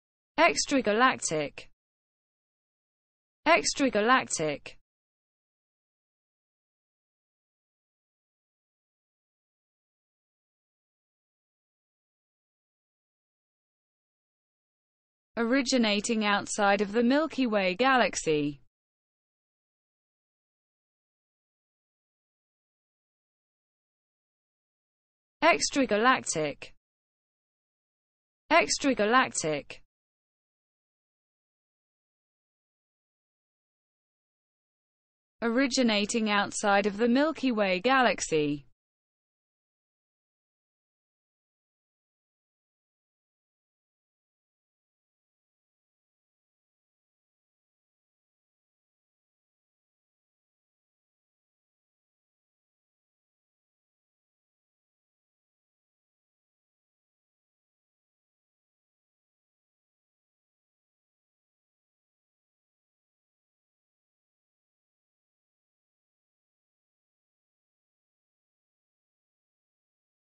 What does Extragalactic mean?
What does Extragalactic mean in English? Learn the meaning of the word Extragalactic! How to Pronounce Extragalactic. Expand your vocabulary, learn words.